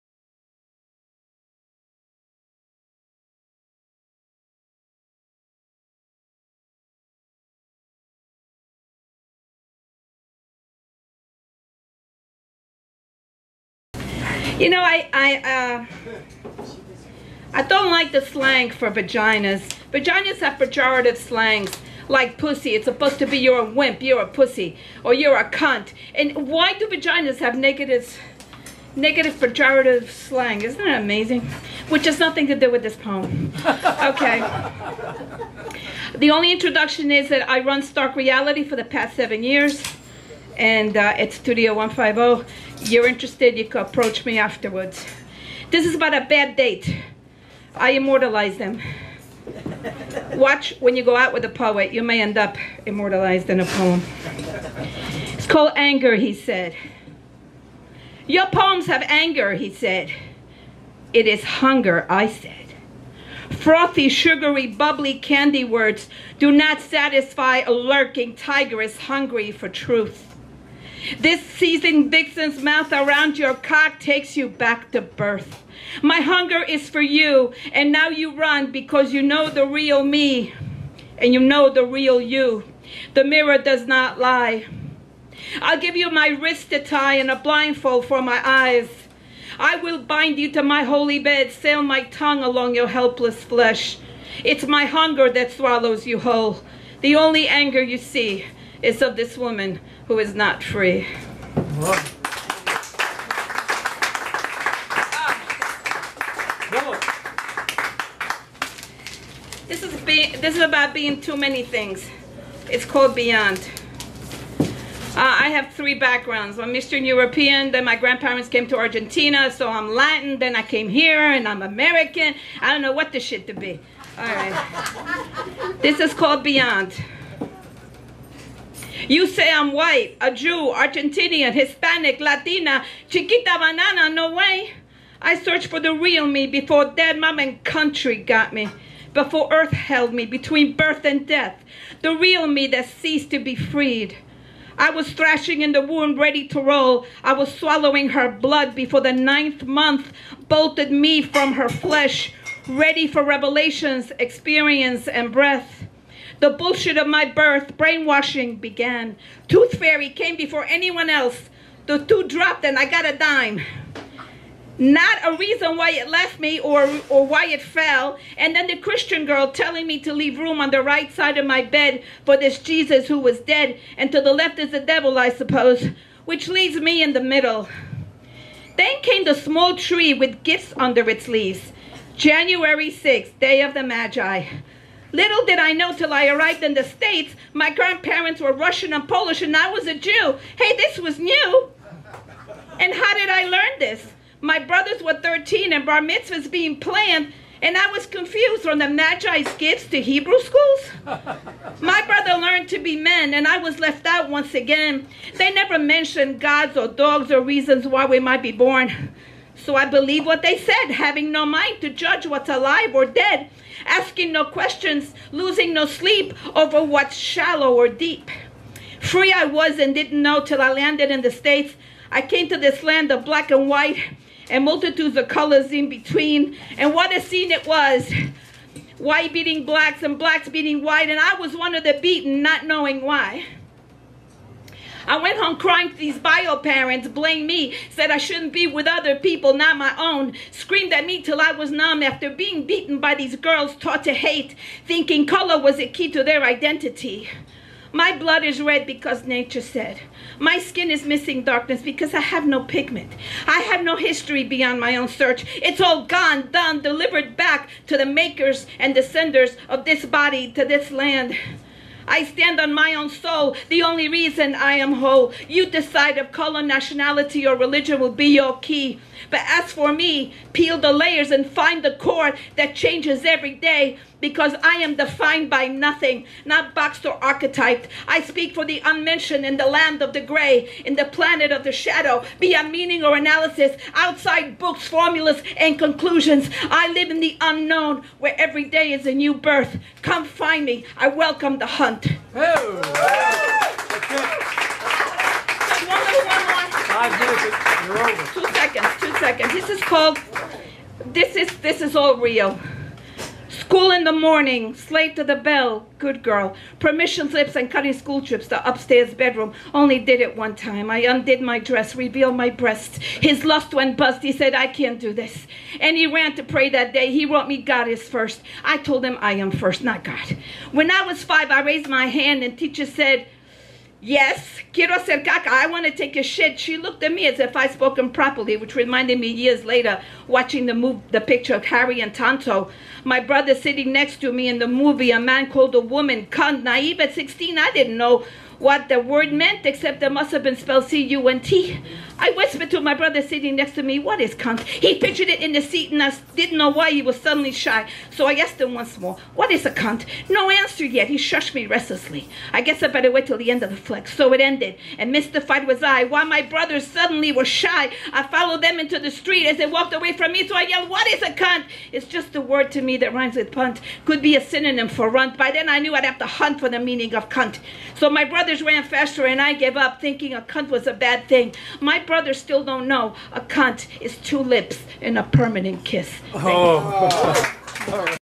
"Open Mic Gypsy" Viviana Grell for Rimes of The Ancient Mariner August 27, 2014
"Open Mic Gypsy" Viviana Grell for Rimes of The Ancient Mariner August 27, 2014 @ Three of Cups Lounge.